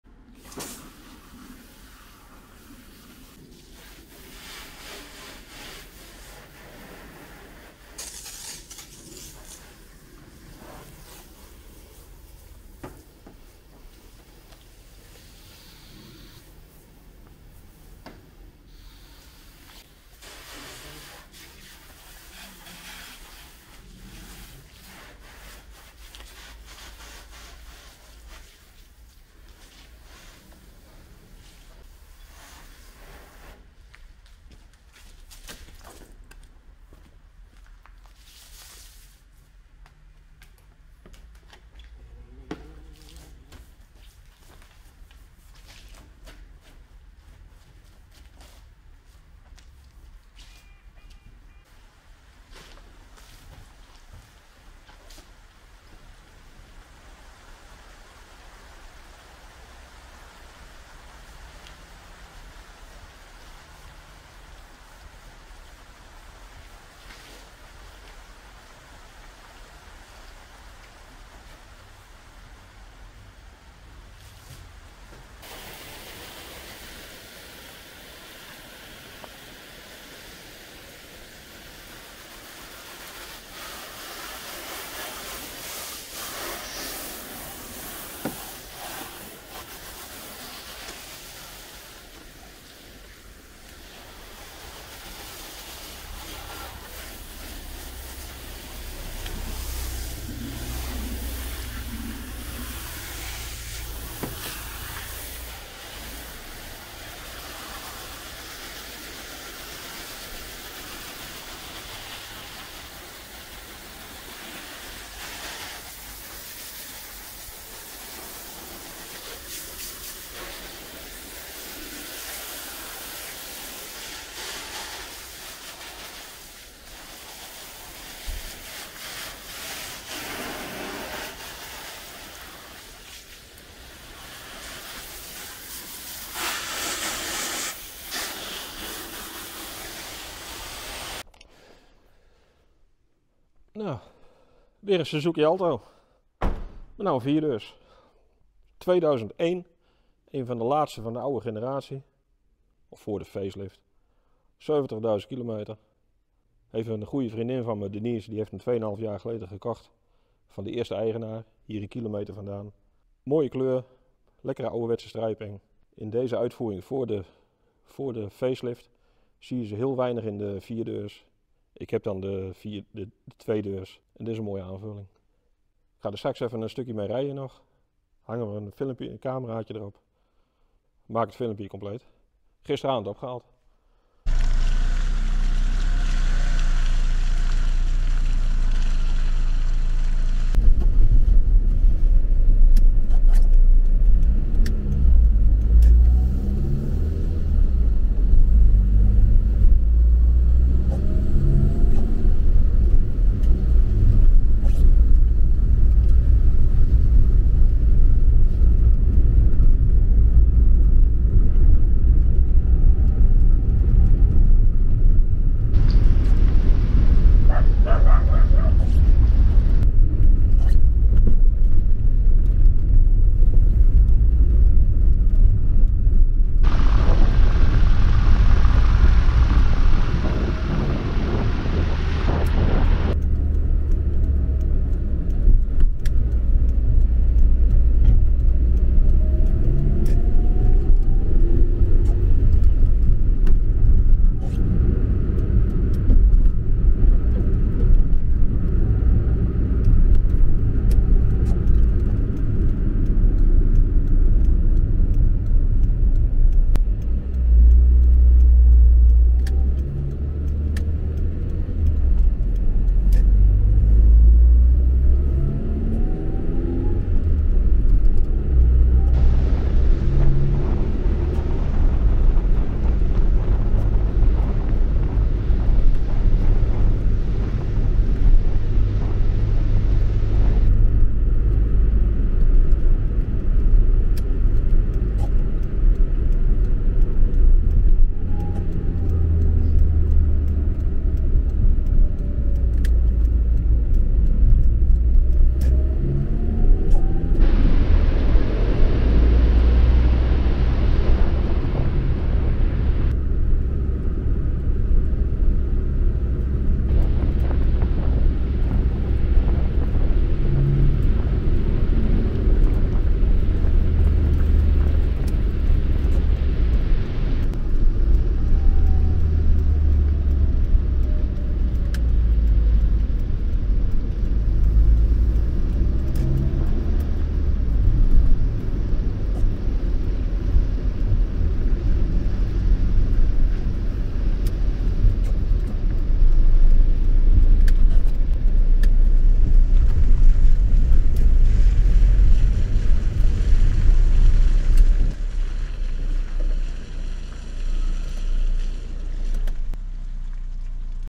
De Suzuki Alto GLS van Denise. Wassen en rijden
2,5 jaar geleden kocht Denise, een hele lieve vriendin van mij deze Suzuki Alto uit 2001. Ze zocht een zuinig autootje om mee naar haar paarden te kunnen rijden. Ze heeft een bestelauto voor haar werk maar die wou ze sparen vanwege het hogere verbruik en kilometers. Deze vond ze op marktplaats op spuugafstand hier in Apeldoorn, zelf woont ze onder de rook van Utrecht.

Op een vrijdagmiddag is ze met de trein naar Apeldoorn gekomen en samen zijn we bij de Alto gaan kijken. De kleinzoon verkocht de auto van zijn oma die hem nieuw had gekocht bij Suzuki dealer Pijnappel alhier. Na een rondje te hebben gereden was Denise er snel uit en ze betaalde en we zetten de auto op haar naam.

Ze heeft er een heerlijke auto aan gehad maar hij werd overbodig. Ze kocht een versere VW Caddy die een stuk zuiniger was en met een frissere kilometerstand. Of ik haar Alto wou adopteren? Nou graag natuurlijk, en zo kreeg ik deze van haar gedoneerd! Deze generatie alto beginnen al langzaam uit het straatbeeld te verdwijnen, in de 4/5 deurs al helemaal. Dus gaan we deze ook weer het zondagse pak aantrekken, uiteraard inclusief die heerlijke old skool striping